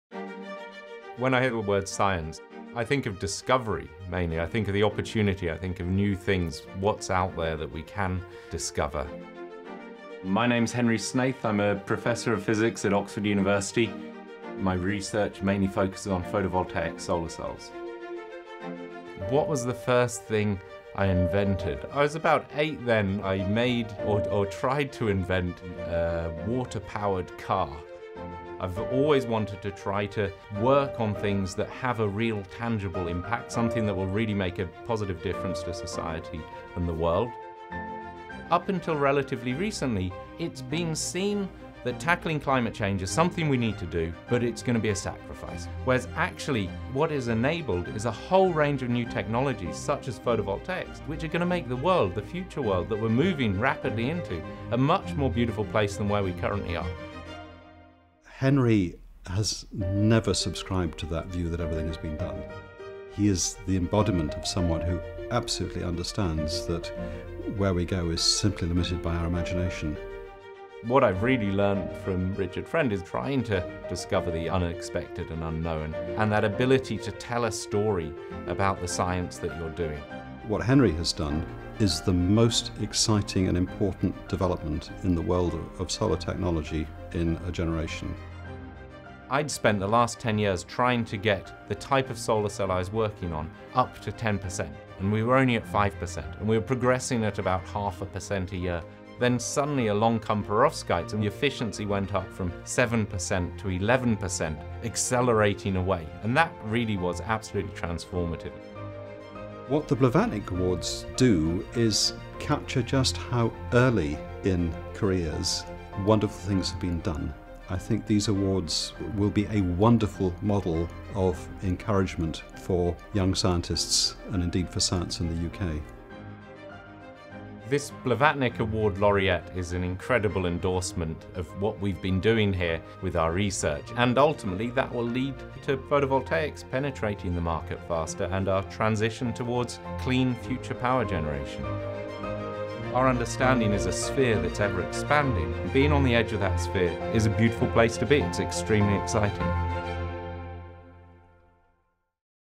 Meet Prof. Henry Snaith, 2018 Blavatnik Awards in the UK Life Sciences Laureate
Meet Inaugural Blavatnik Awards in the UK, 2018 Laureate in Physical Sciences and Engineering, Prof. Henry Snaith, a Professor of Physics at the University of Oxford. Prof. Snaith works in the field of photovoltaics, pioneering new, low-cost and high-efficiency solar cells based on metal halide perovskite materials, a material previously overlooked. Prof. Snaith has not only initiated a new research field now studied by scientists around the world, but his work also has the potential to deliver solar energy to the market at a fraction of the cost of currently used materials.